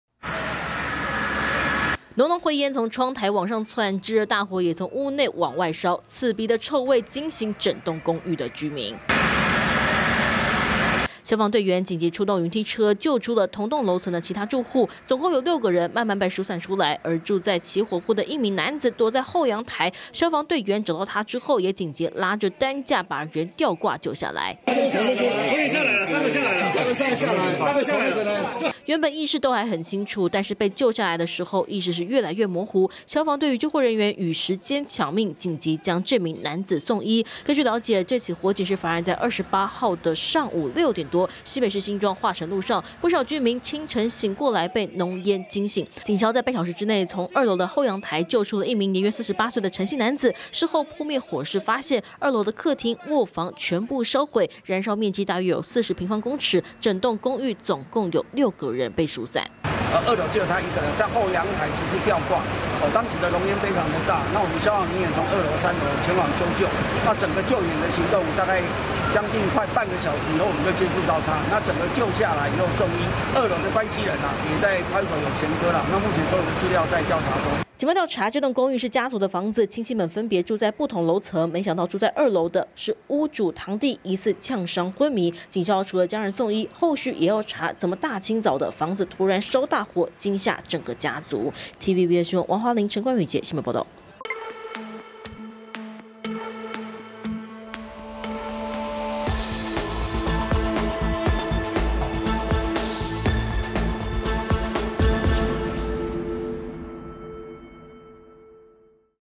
公寓竄濃灰煙 躲2F後陽台獲救 住戶昏迷｜TVBS新聞
清晨六點多，新北市新莊一間公寓竄出大火，警消出動雲梯車救援，疏散六名住戶，並將其中一名，一度受困的男子緊急送醫！警消調查，這棟公寓住著一整個家族，親戚們分別住在不同樓層，沒想到這回大清早，二樓竄出火舌，屋主堂弟送醫後意識不清。